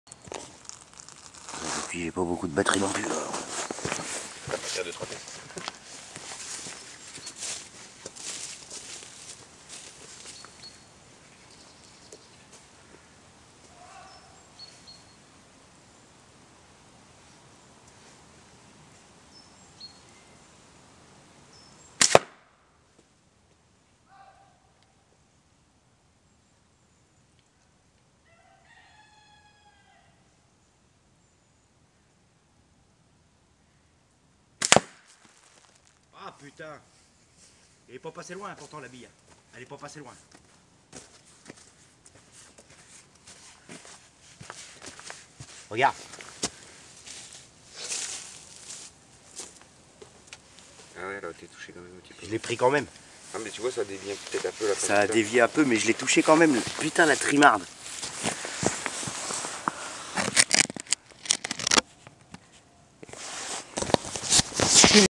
lance pierre ebs
tir ballon et carte , presque réussi  !!! mdr